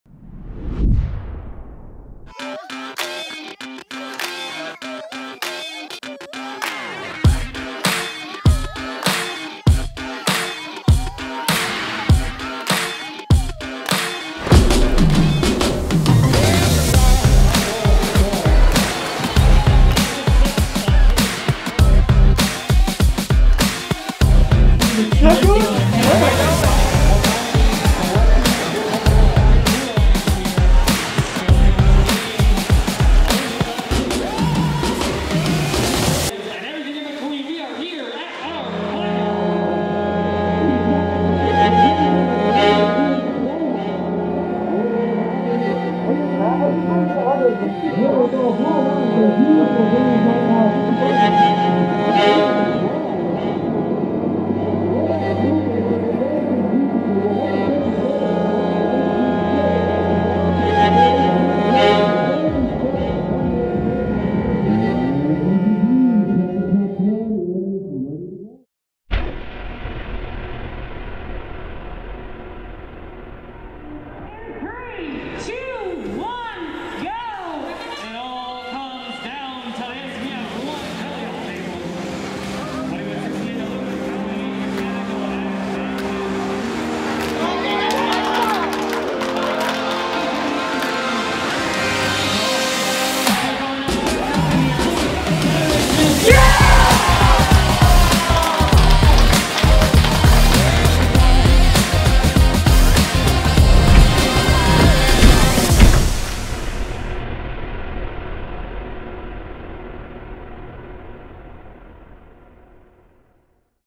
FRC 6328 BattleCry 2021 Wrap Video
We can’t thank everyone involved with BattleCry 2021 enough for hosting such a safe and fun event. Special shout out to our alliance partners 2168, 2262, and 1735 for forming such a collaborative and exciting alliance, and helping us take home a gold medal!